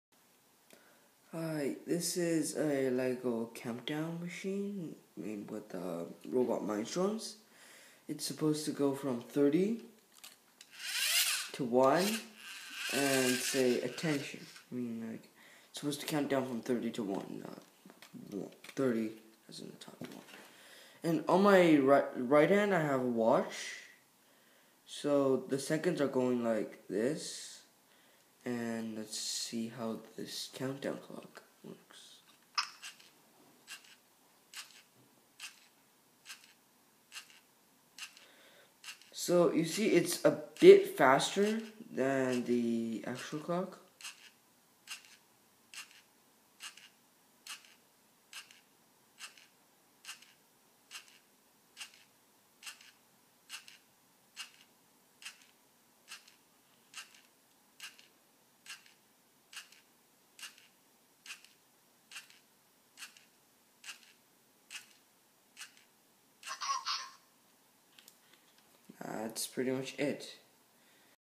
Mind storms count down machine
A machine that counts down from 30 to 1